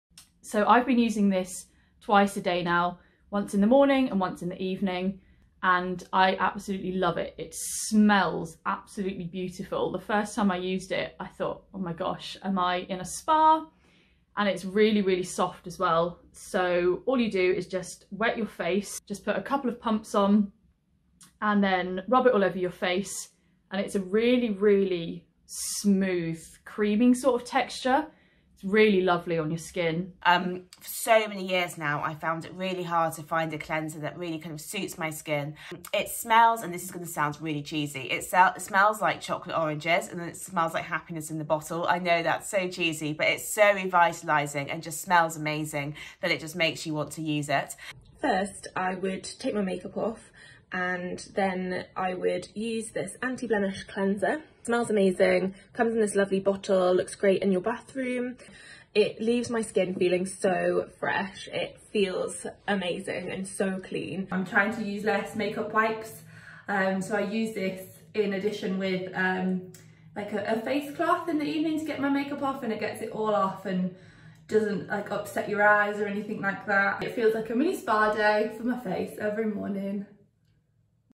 Reviews of 47 Skin Everyday Cleanser
47 Skin customers review our Anti-Blemish & Scar Repair Everyday cleanser.